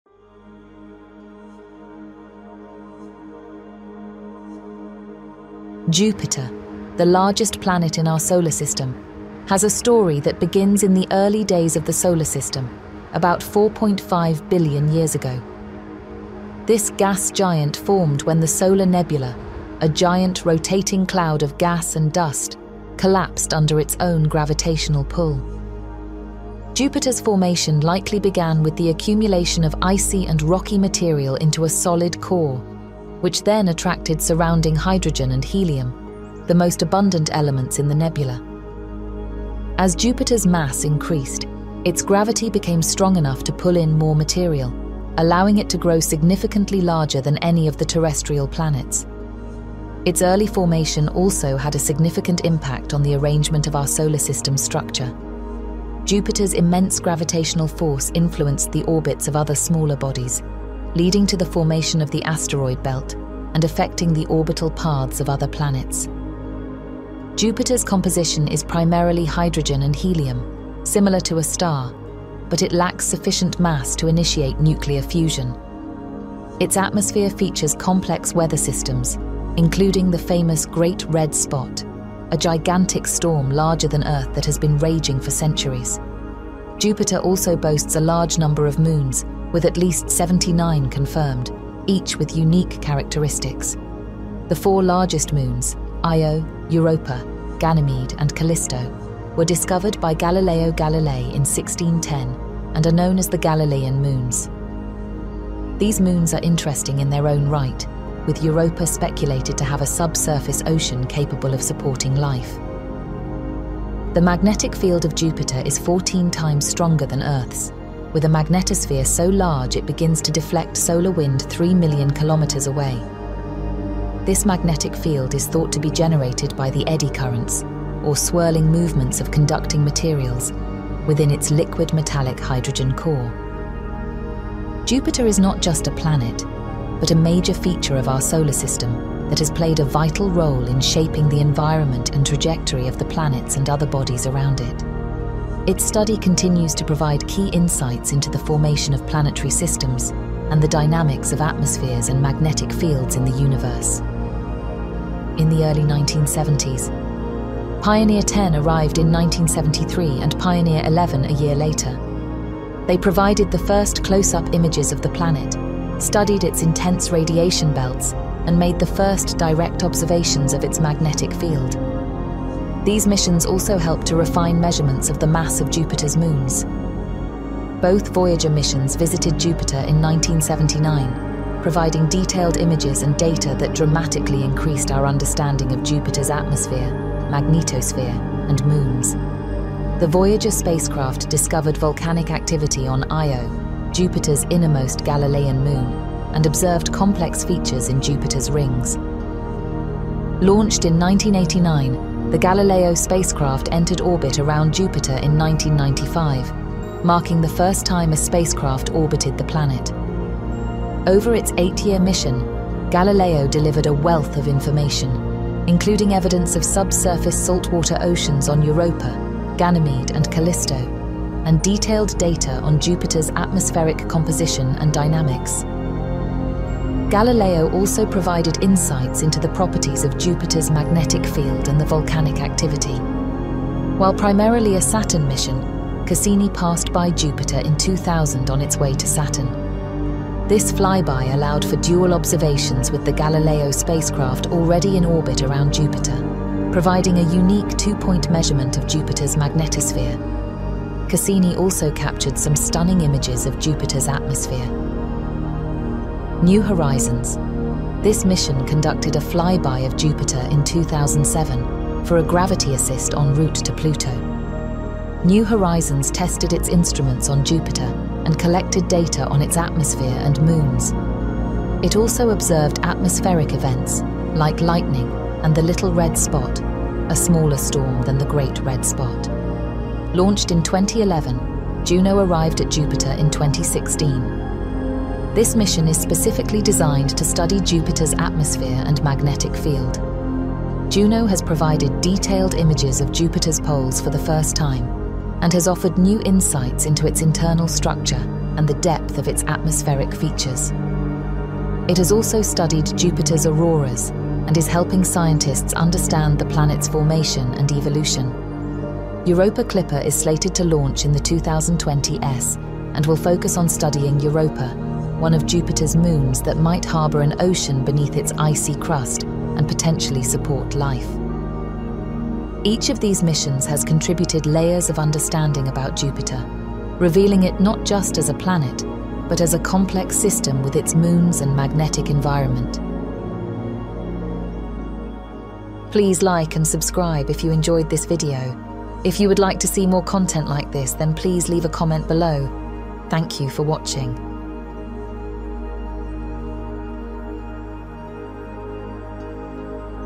Exploring Jupiter: A Journey Through the Gas Giant's Atmosphere
Exploring Jupiter: A Journey Through the Gas Giant's Atmosphere. The largest planet in our solar system, has a story that begins in the early days of the solar system, about 4.5 billion years ago. This gas giant formed when the solar nebula, a giant rotating cloud of gas and dust, collapsed under its own gravitational pull. Jupiter's formation likely began with the accumulation of icy and rocky material into a solid core, which then attracted surrounding hydrogen and helium, the most abundant elements in the nebula.

As Jupiter's mass increased, its gravity became strong enough to pull in more material, allowing it to grow significantly larger than any of the terrestrial planets. Its early formation also had a significant impact on the arrangement of our solar system’s structure. Jupiter's immense gravitational force influenced the orbits of other smaller bodies, leading to the formation of the asteroid belt and affecting the orbital paths of other planets.

Jupiter's composition is primarily hydrogen and helium, similar to a star, but it lacks sufficient mass to initiate nuclear fusion. Its atmosphere features complex weather systems, including the famous Great Red Spot, a gigantic storm larger than Earth that has been raging for centuries.

Jupiter also boasts a large number of moons, with at least 79 confirmed, each with unique characteristics. The four largest moons—Io, Europa, Ganymede, and Callisto—were discovered by Galileo Galilei in 1610 and are known as the Galilean moons. These moons are interesting in their own right, with Europa speculated to have a subsurface ocean capable of supporting life.

The magnetic field of Jupiter is fourteen times stronger than Earth's, with a magnetosphere so large it begins to deflect solar wind 3 million kilometers away. This magnetic field is thought to be generated by the eddy currents, or swirling movements of conducting materials, within its liquid metallic hydrogen core.

In conclusion, Jupiter is not just a planet, but a major feature of our solar system that has played a vital role in shaping the environment and trajectory of the planets and other bodies around it. Its study continues to provide key insights into the formation of planetary systems and the dynamics of atmospheres and magnetic fields in the universe.

This essay provides a comprehensive look at Jupiter, from its formation to its current status and influence within our solar system. As an animator focused on space science, you could create a visually engaging animation that brings this narrative to life, highlighting Jupiter's dynamic features and historical impact on our solar system's evolution.